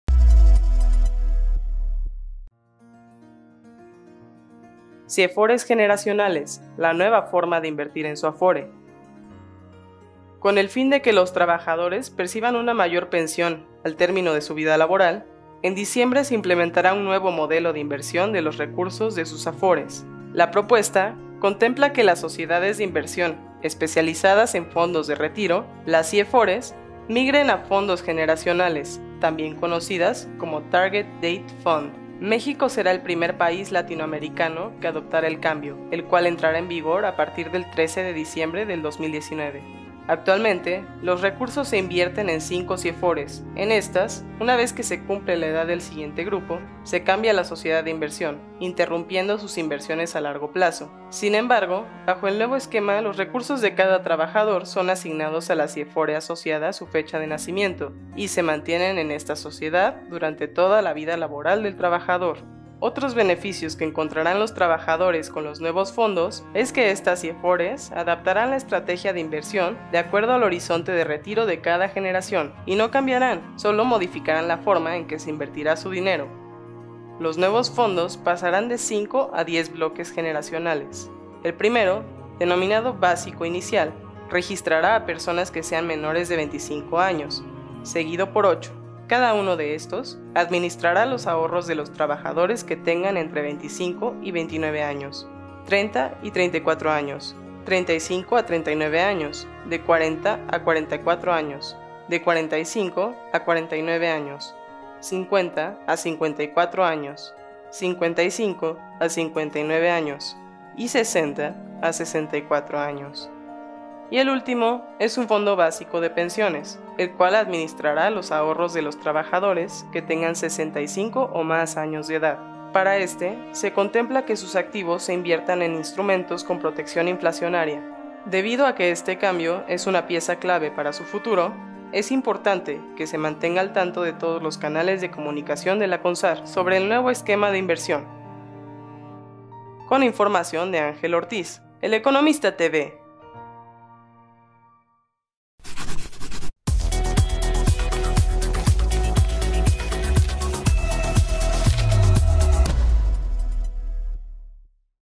Siefores generacionales, la nueva forma de invertir en su afore
Los trabajadores contarán con una nueva estrategia de inversión de acuerdo al horizonte de retiro de cada generación.